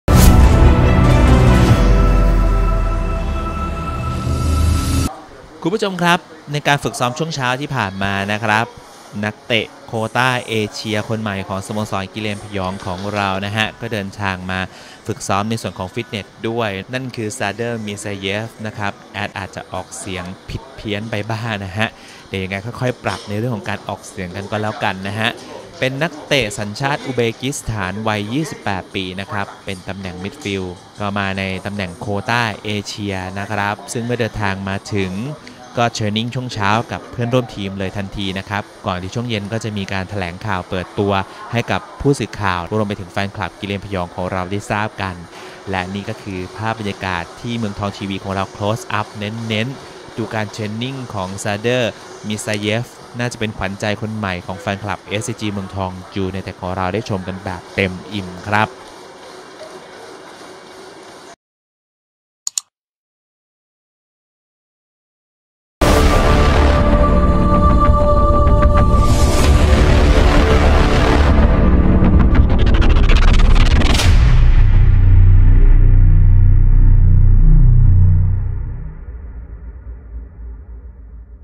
ซาร์ดอร์ มีร์ซาเยฟ ดาวเตะทีมชาติอุซเบกิสถาน ฝึกซ้อมกับเพื่อนร่วมทีมครั้งแรก
เมืองทองทีวี : ซาร์ดอร์ มีร์ซาเยฟ นักเตะใหม่สโมสรเอสซีจีเมืองทองฯ ( หมายเลข 10 ) ดาวเตะทีมชาติอุซเบกิสถาน ฝึกซ้อมกับเพื่อนร่วมทีมครั้งแรก